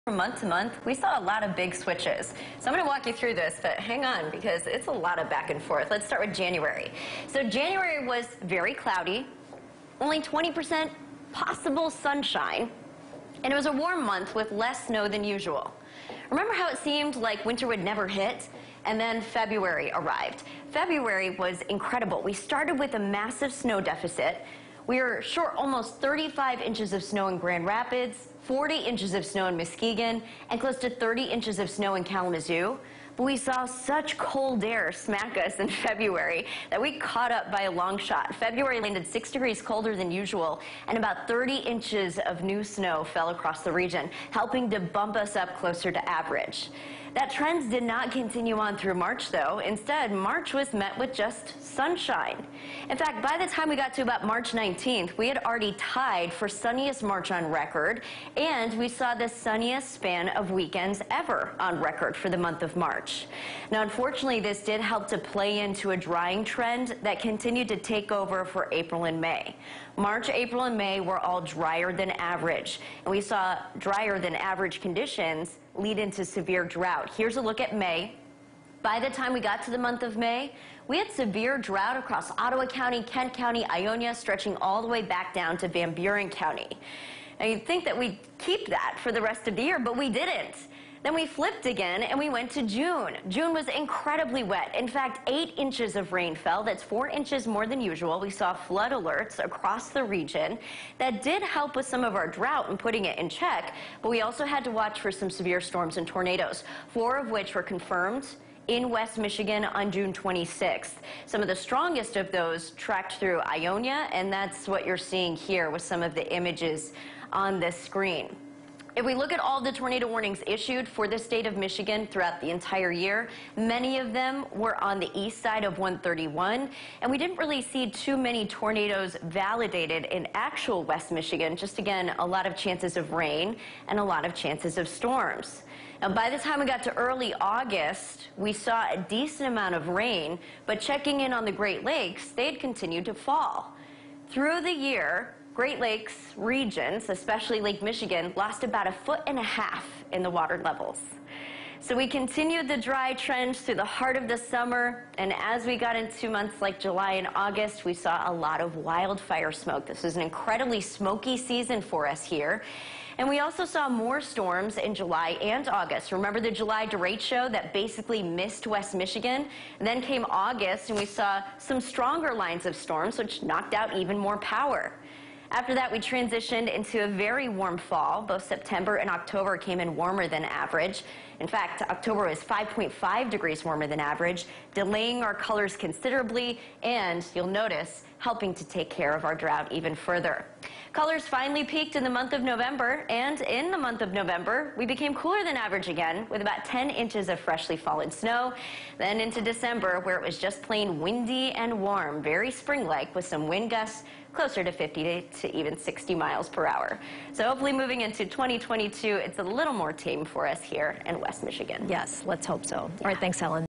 2021 Year in Review: Weather
The weather sure threw West Michigan some punches in 2021. Here are the biggest take-aways on our year in review.